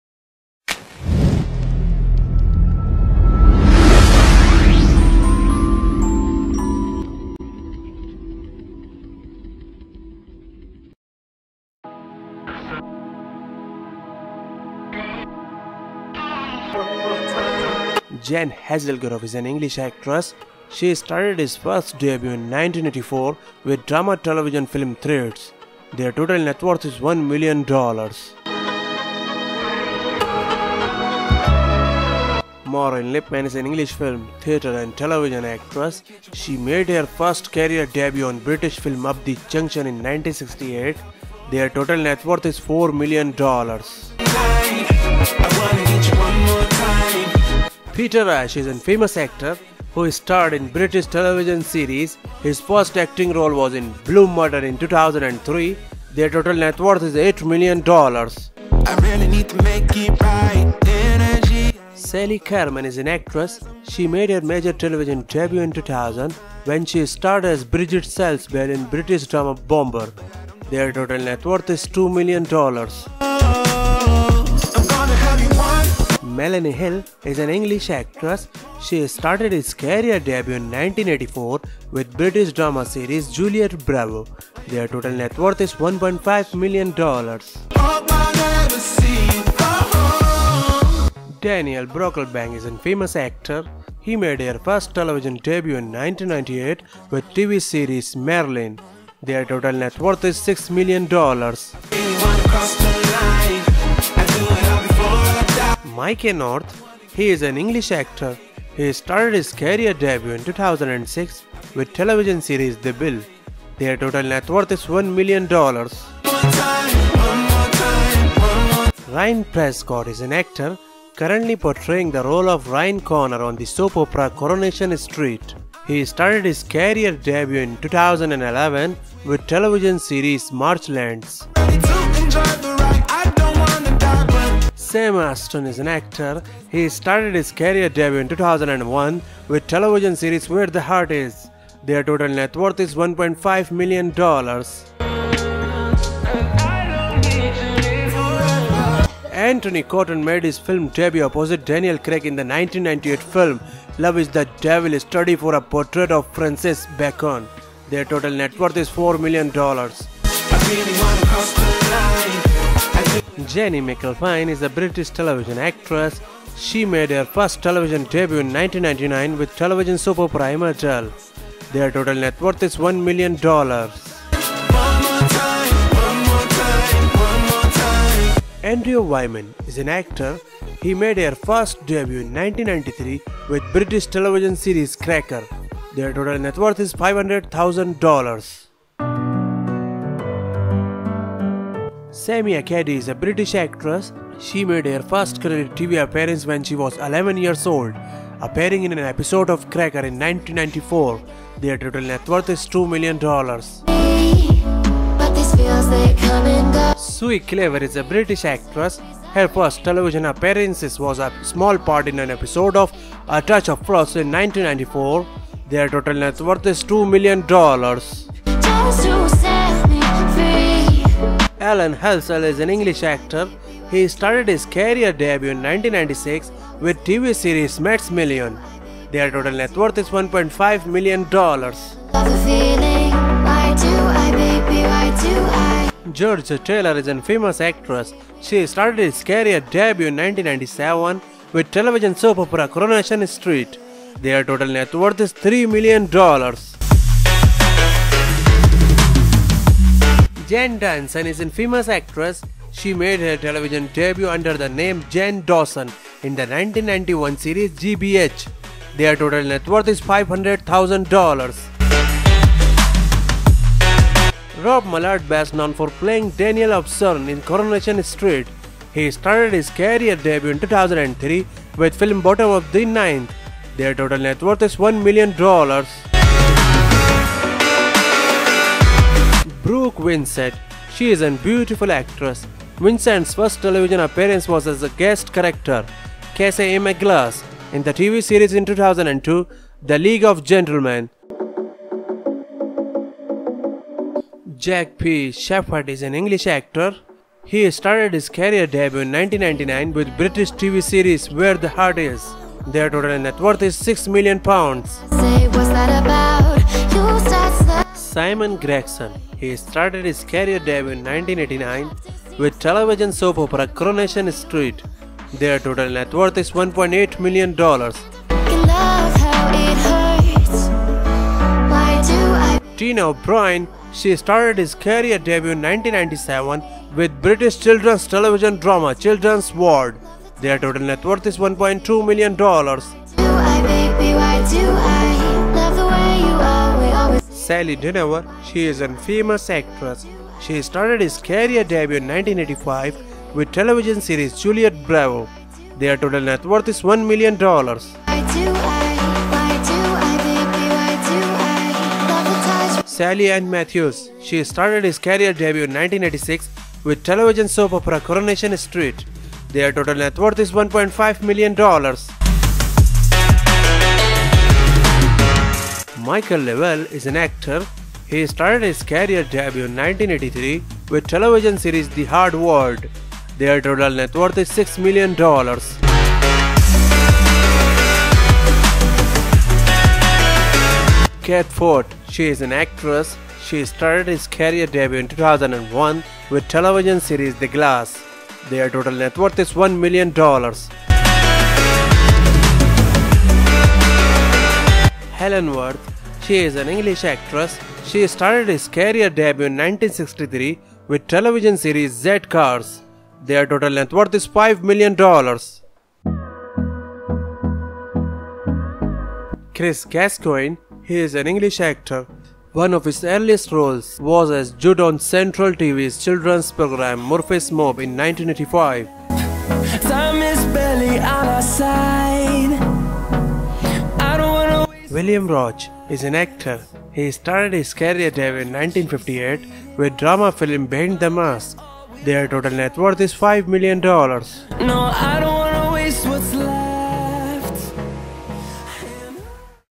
Coronation Street Net Worth & Debut || Soap Opera Salary
Coronation Street is a British television Soap opera. In this video i will showing Coronation Street salaries, net worth, and first debut of all cast and characters. I have watch some of Coronation Street full episodes. Highest paid actors of soap operas.

CAST (Actors)
William Roache
Barbara Knox
Chris Gascoyne
Helen Worth
Kate Ford
Sue Nicholls
Ben Price
Michael Le Vell
Sally Dynevor
Tina O'Brien
Beverley Callard
Simon Gregson
Jack P. Shepherd
Malcolm Hebden
Brooke Vincent
Rob Mallard
David Neilson
Jane Danson
Georgia Taylor
Alan Halsall
Jimmi Harkishin
Sue Cleaver
Sam Robertson
Jennie McAlpine and others